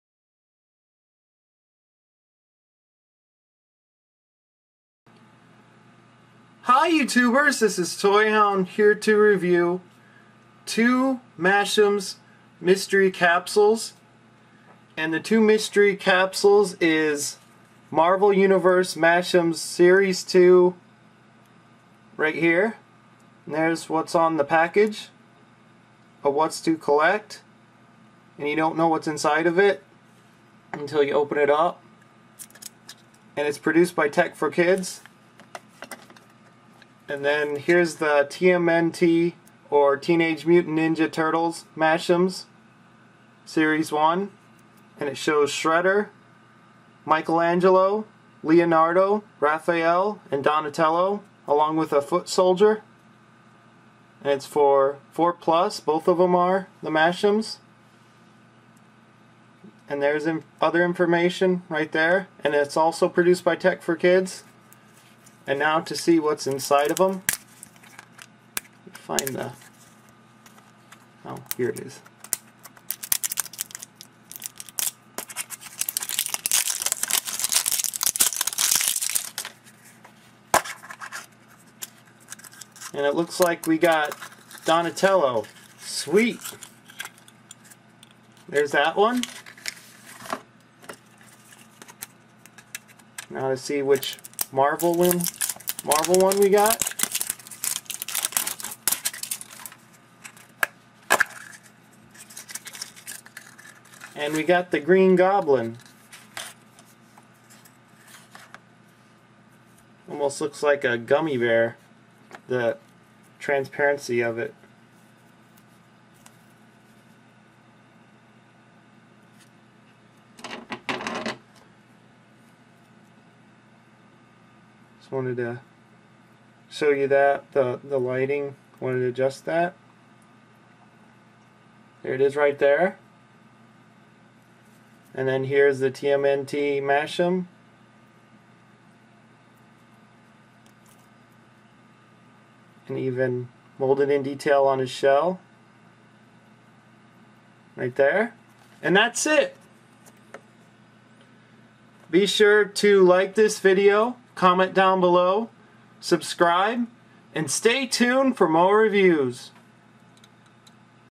Toyhound review,MARVEL and TMNT MASH'EMS CAPSULE Opening
Marvel Universe Mash'ems and TMNT Mash'ems for Ages 4+

In this video is a review of two Mash'ems Mystery capsules, one is a Marvel Universe Series 2 Mashem and a Teenage Mutant Ninja Turtles Mashem.

I found and purchased both theses Mash'em Capsules at Toysrus retail store for $2.99.

Here is links to Tech4kids toy company website and its mashems toy brand website posted below